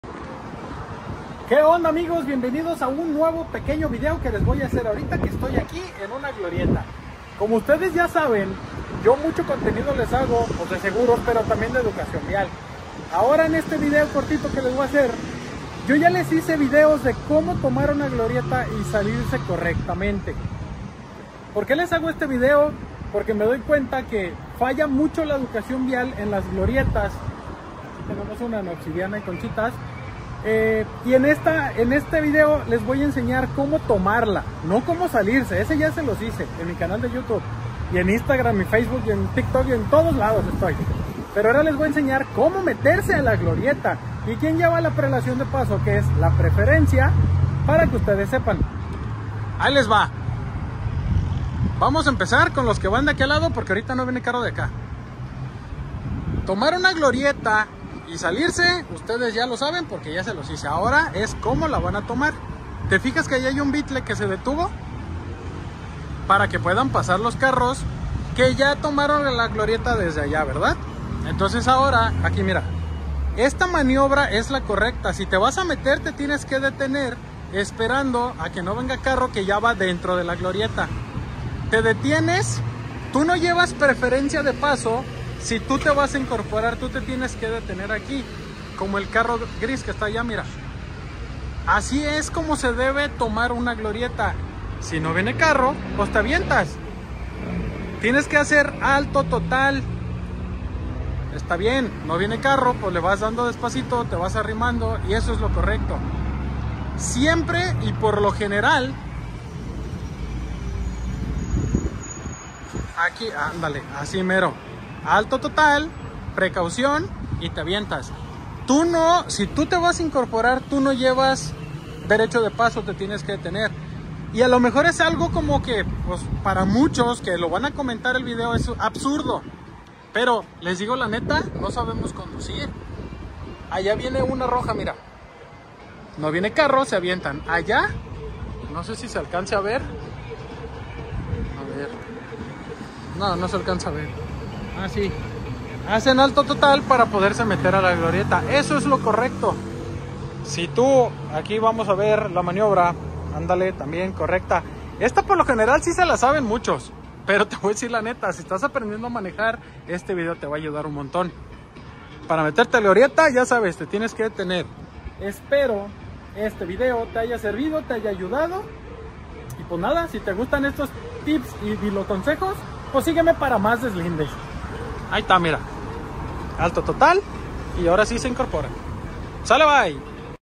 ¡Manejar en glorietas! No es para cualquier conductor
Si no conoces el reglamento vial por favor no manejes! Cuídate y cuida a los demás conductores